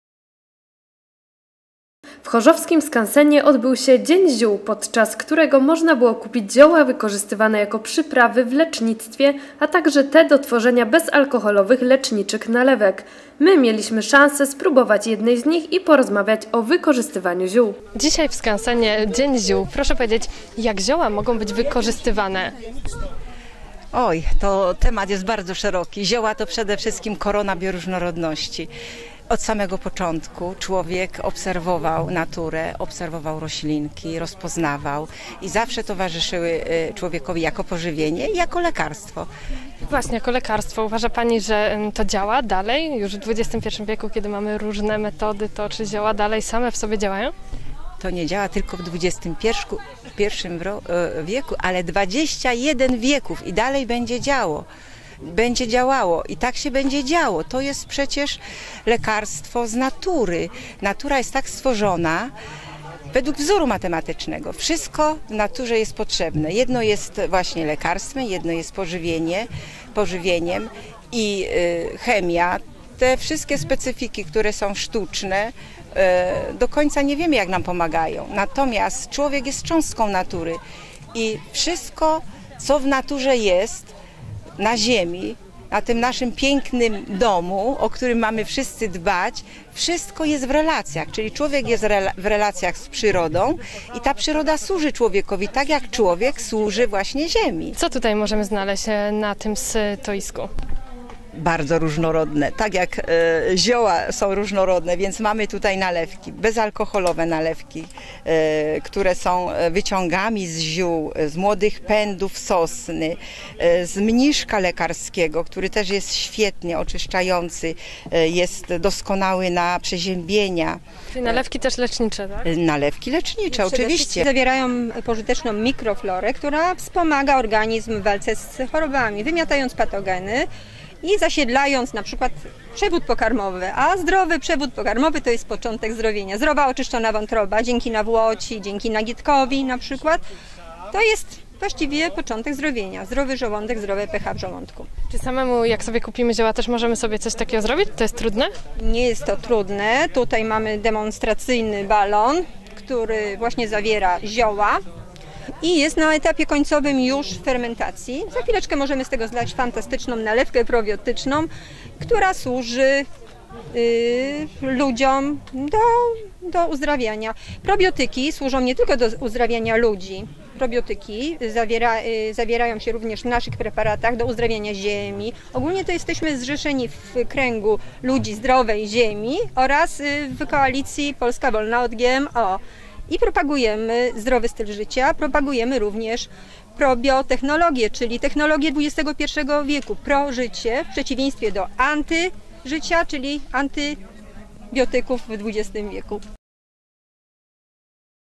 Dzień Ziół w chorzowskim skansenie
W chorzowskim Skansenie odbył się Dzień Ziół, podczas którego można było kupić zioła wykorzystywane jako przyprawy, w lecznictwie, a także te do tworzenia bezalkoholowych, leczniczych nalewek. My mieliśmy szansę spróbować jednej z nich i porozmawiać o wykorzystaniu ziół.